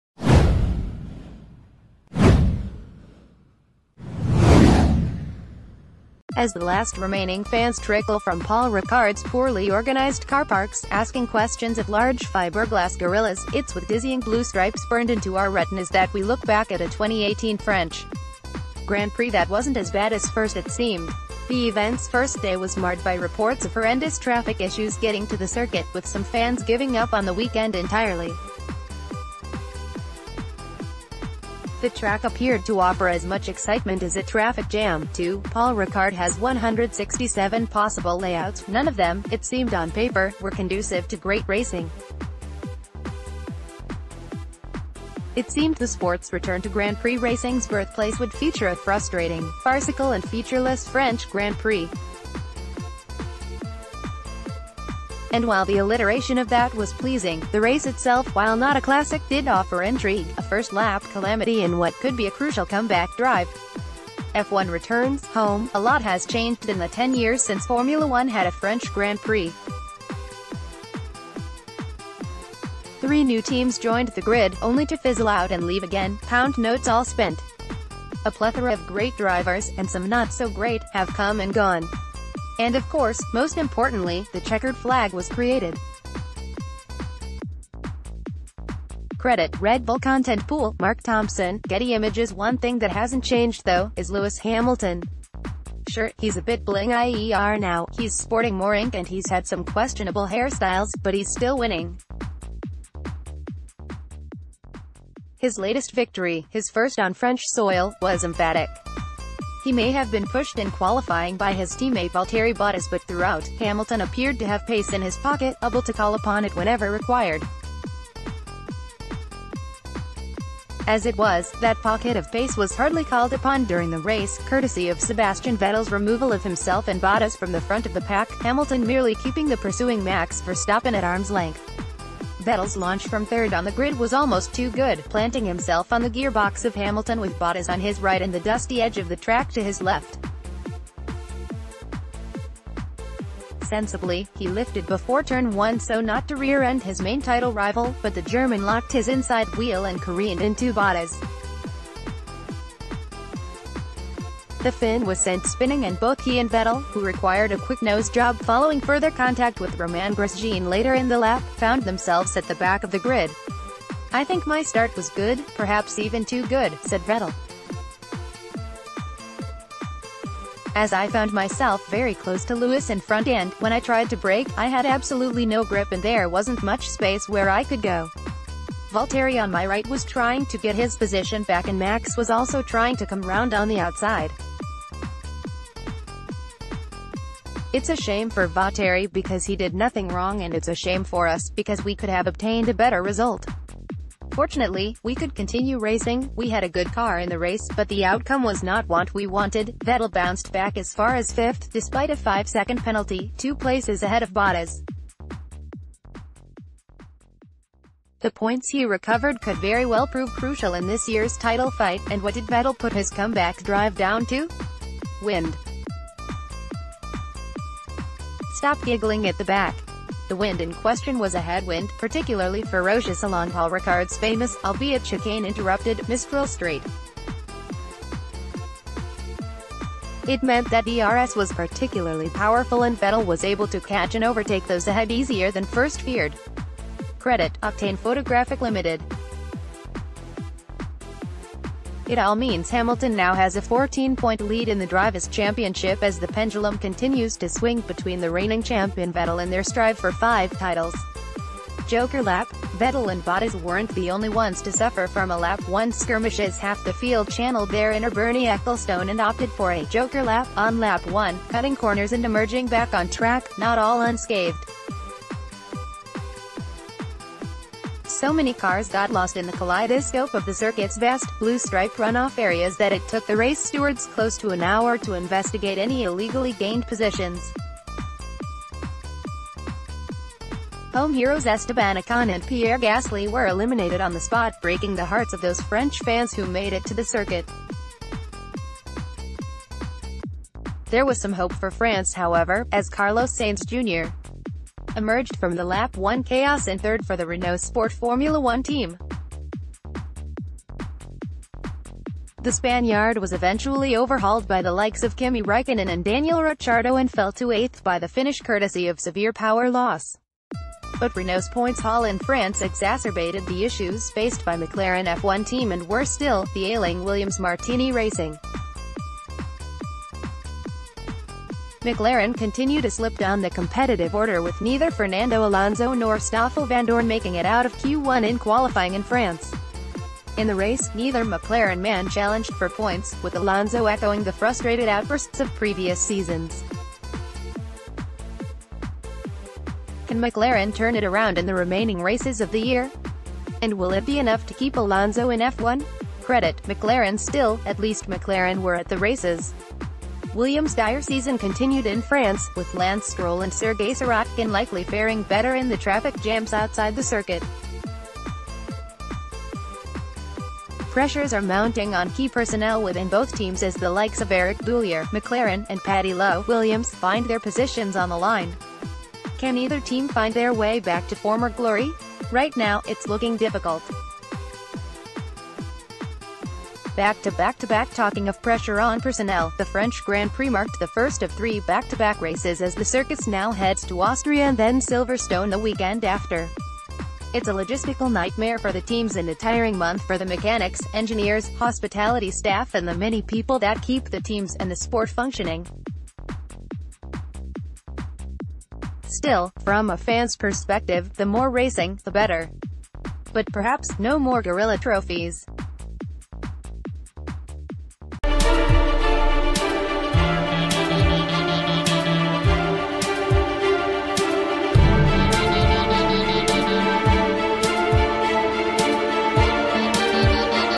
2018 French Grand Prix: Analysis
2018 French Grand Prix: Analysis
2018 French Grand Prix: Analysis

As the last remaining fans trickle from Paul Ricard’s poorly organised car parks, asking questions of large fibreglass gorillas, it’s with dizzying blue stripes burned into our retinas that we look back at a 2018 French Grand Prix that wasn’t as bad as first it seemed… The event’s first day was marred by reports of horrendous traffic issues getting to the circuit, with some fans giving up on the weekend entirely. The track appeared to offer as much excitement as a traffic jam, too; Paul Ricard h...